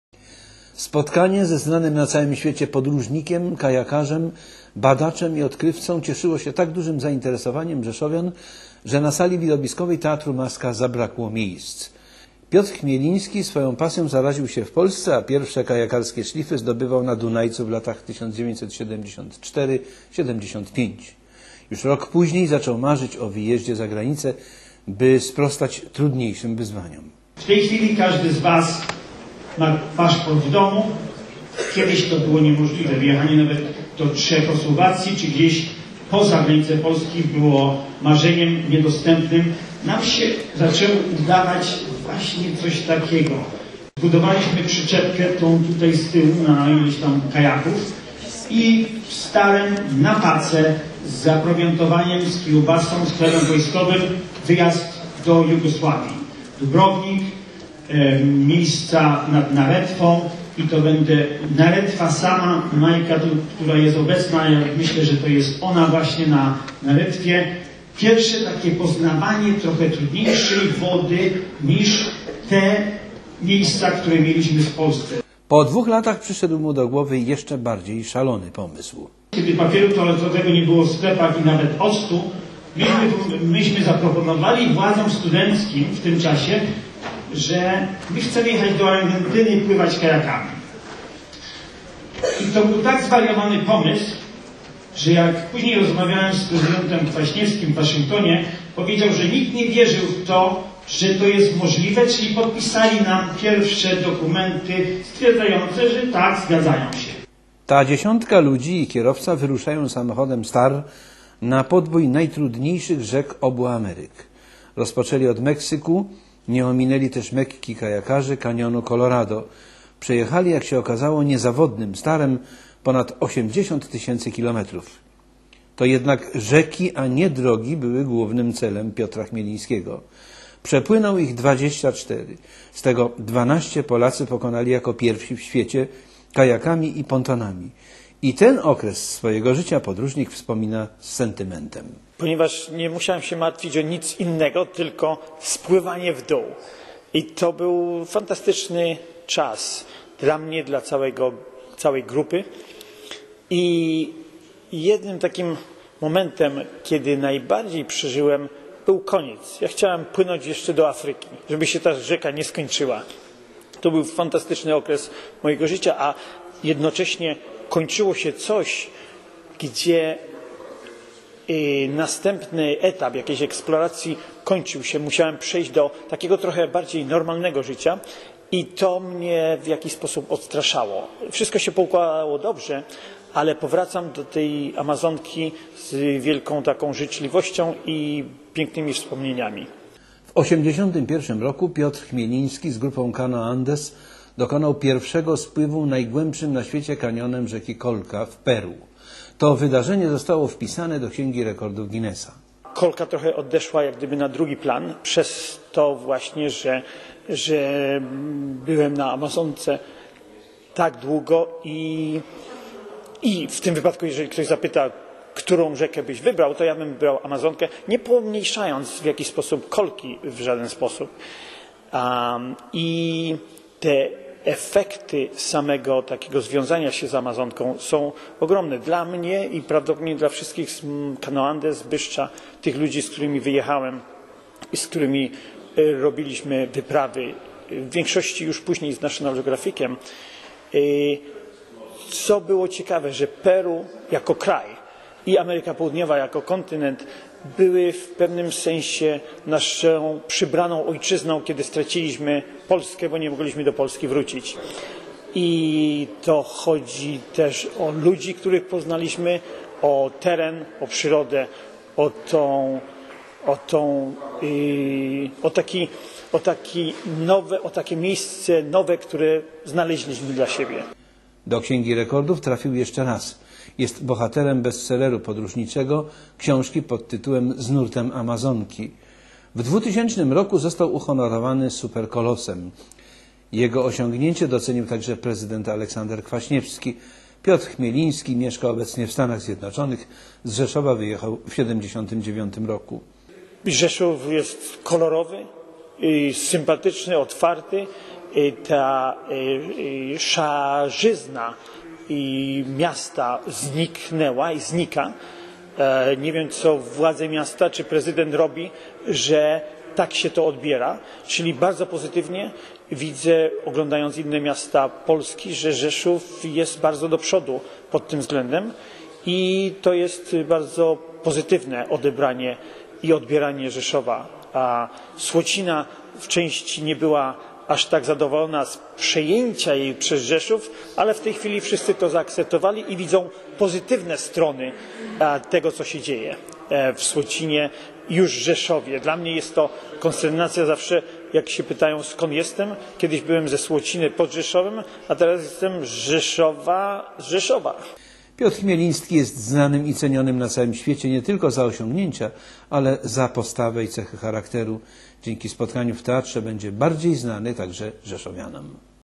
Piotr Chmieliński, Super Kolos (choć średniego wzrostu)
Super Kolos to nagroda, jaką otrzymał za swoje osiągnięcia. A te są niebagatelne: odkrycie najgłębszego wąwozu, wyprawy, rekordy, które dwukrotnie wprowadziły go do Księgi Guinnessa. O swojej podróżniczej pasji opowiadał podczas spotkania w rodzinnym Rzeszowie.